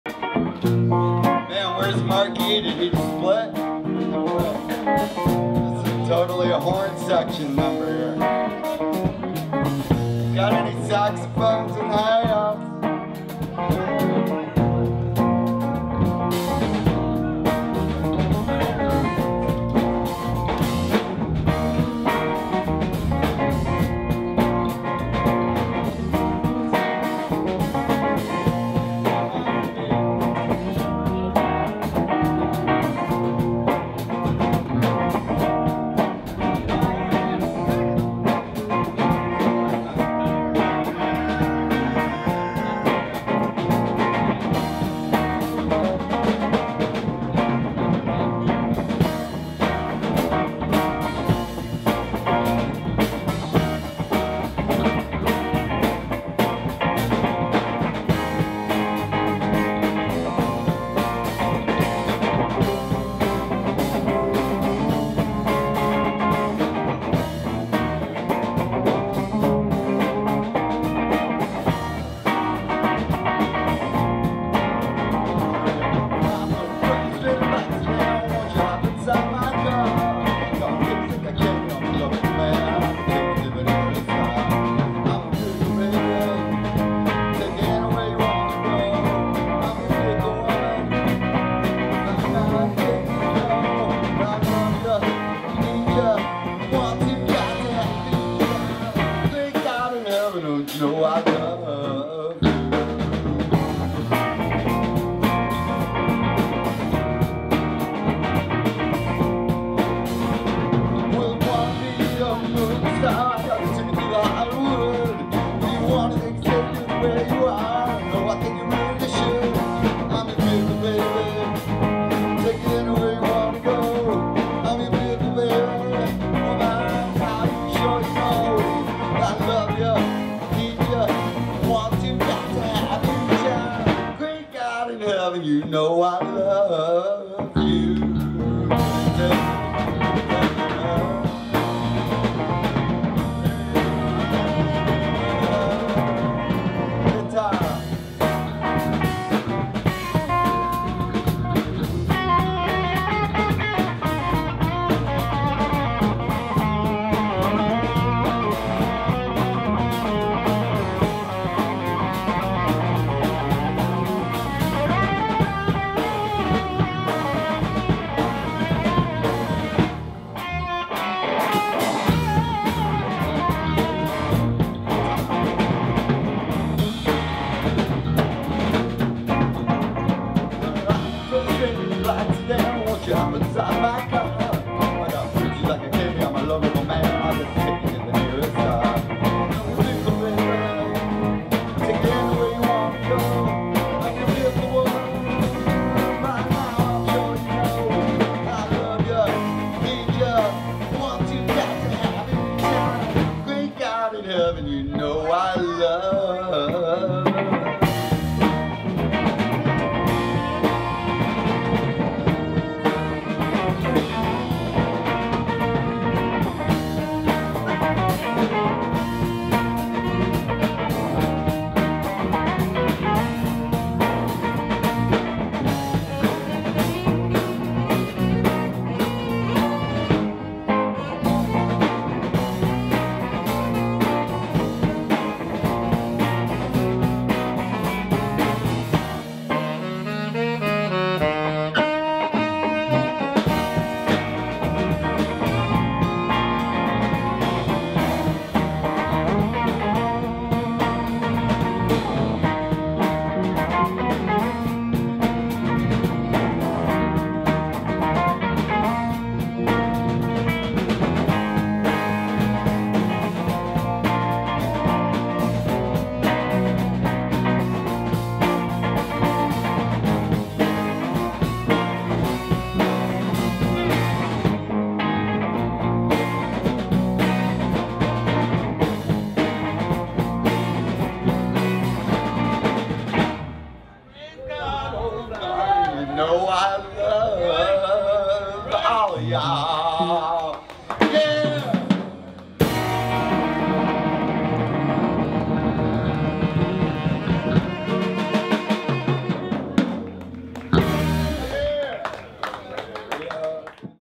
Labor Day at the New Farm - Saturday - Robley Evans 11 - 4K
Three days of live music to celebrate Labor Day at the New Farm SF. Here's the eleventh song on Saturday. We don't own the songwriting copyright.